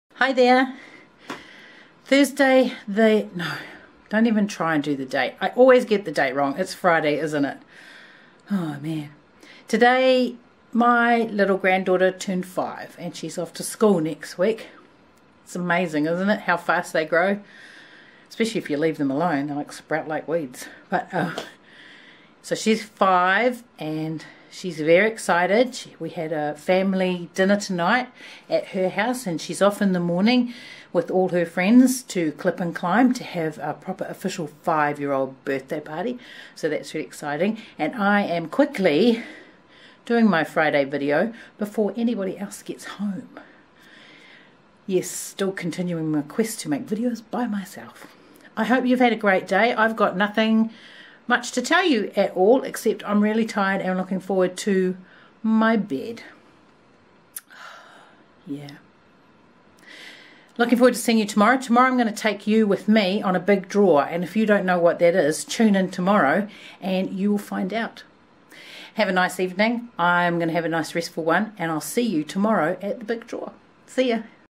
It's quick, it's dirty, it's done (it's not really dirty)
A quick hi and update on a Friday (even though I am still having calendar issues).